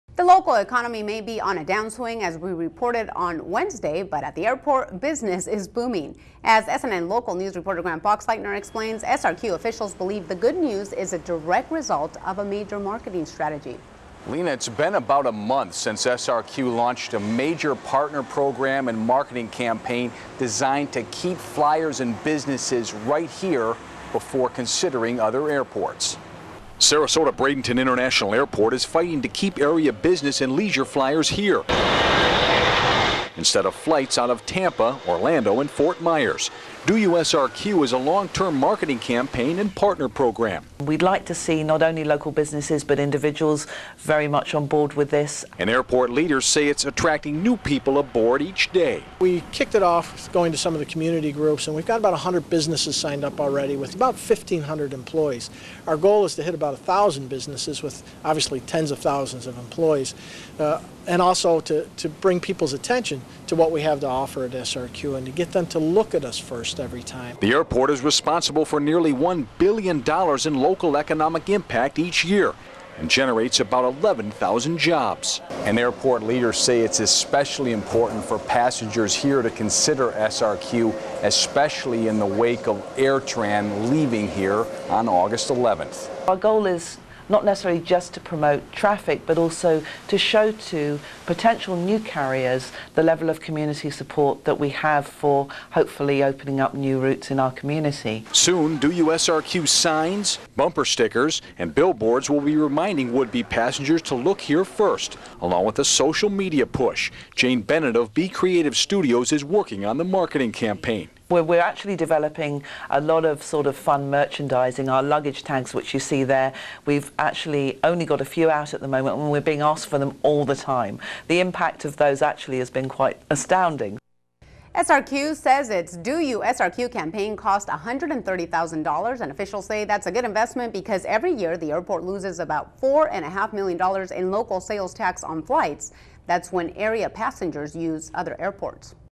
SNN: Economy downsizing, Business Booming, Do You SRQ?
SRQ works hard to keep you flying here.  We take you behind the scenes of a major ad campaign seen all around town.